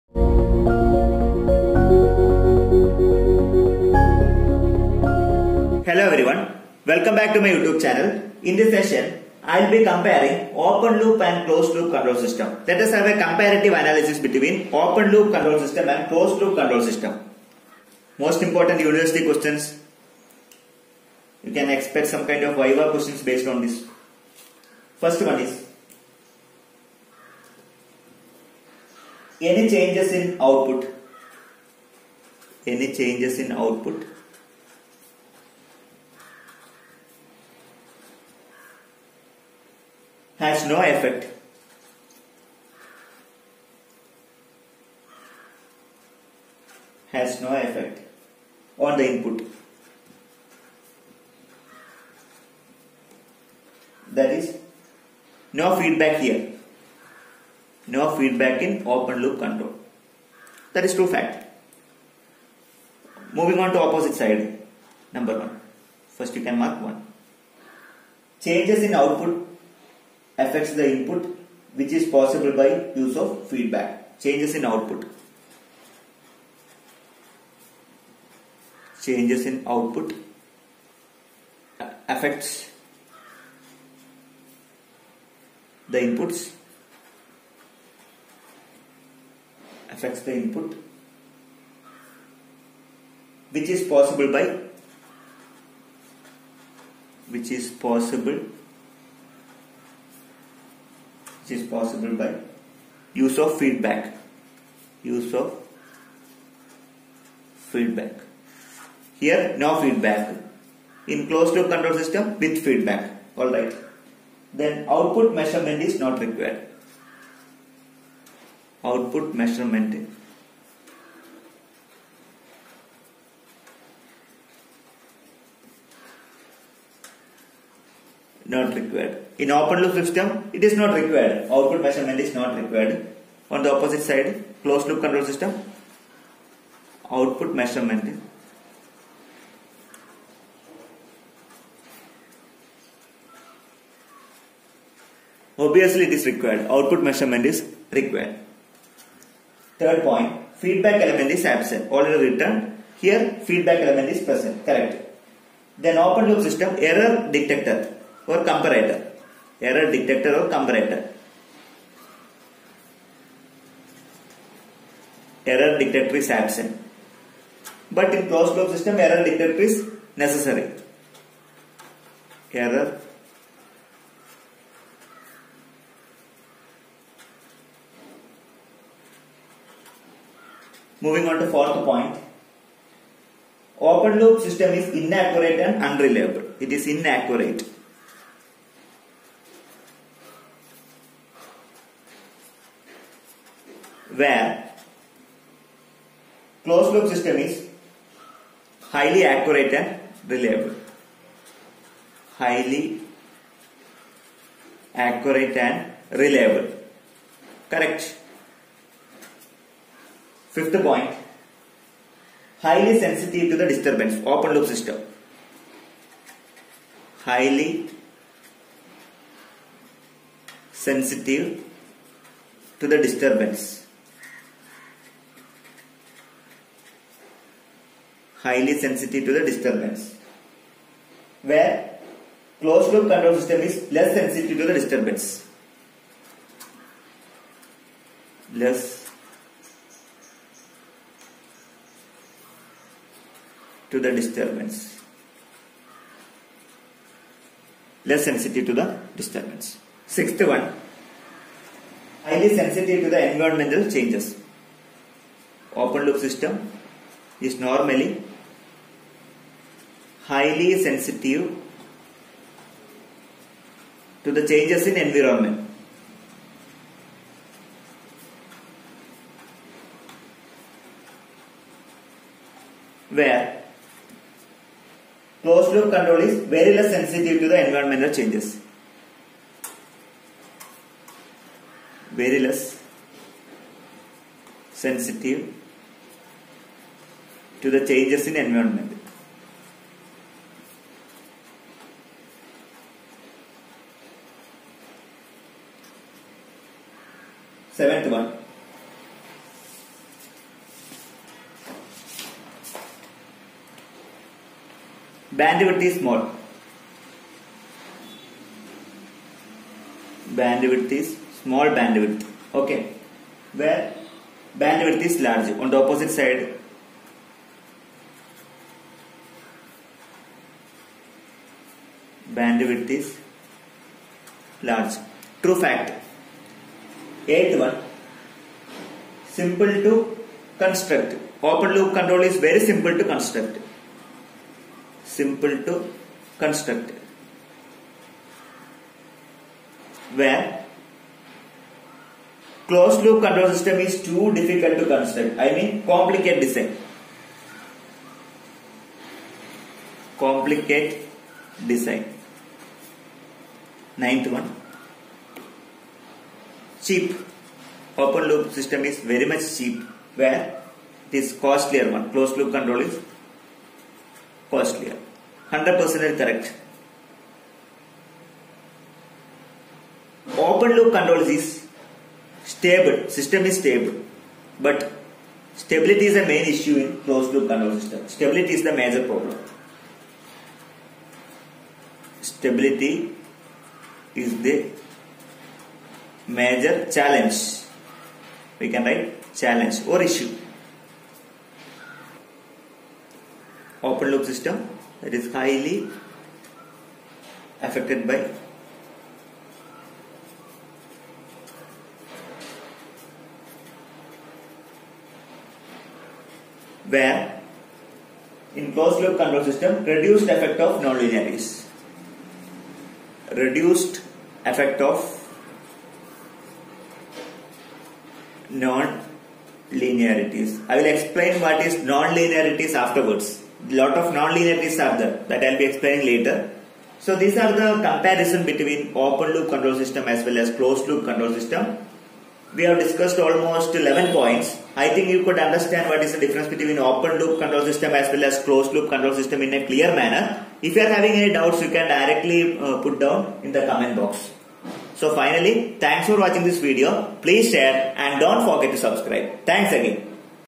Comparison of Open Loop & Closed Loop Control Systems
Hello all,
                 In this video, I have added major comparison between open and closed loop control system.